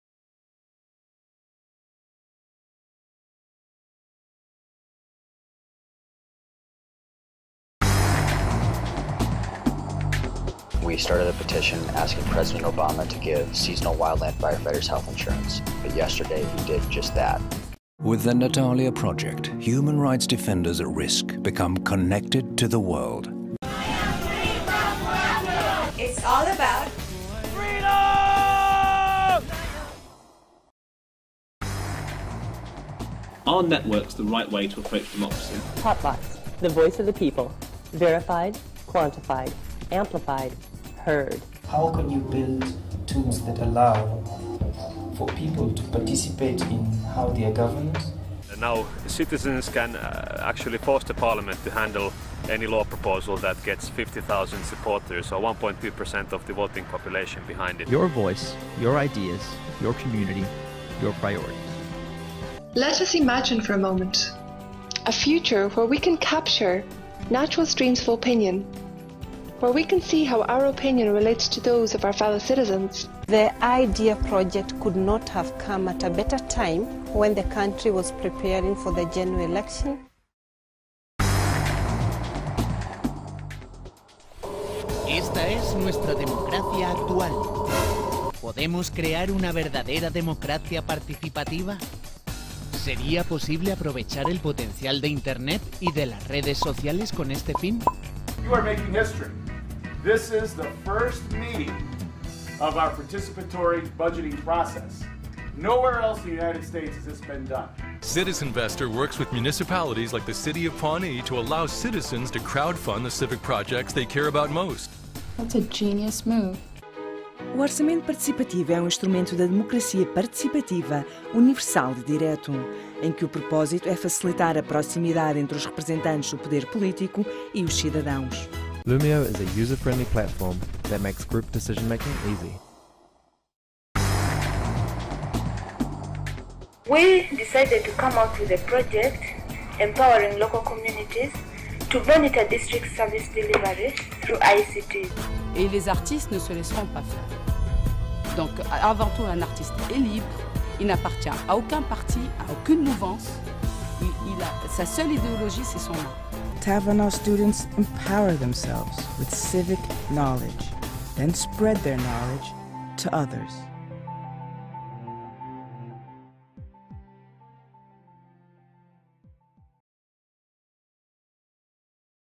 World Forum for Democracy Trailer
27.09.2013 - The World Forum for Democracy with the title "Re-wiring democracy: connecting institutions and citizens in the digital age" is organised by the Council of Europe and its partners and will be held on 23-29 November 2013. It will critically examine the promise of innovative strategies of political participation to remedy the worrying trends of disengagement and lack of trust in modern democracies. In 19 labs, participants from the world of politics, academia, civil society, business, media and social networks will critically review these innovative initiatives, assess their mainstreaming potential, inject new ideas into them, and identify the most promising ones for further promotion and dissemination.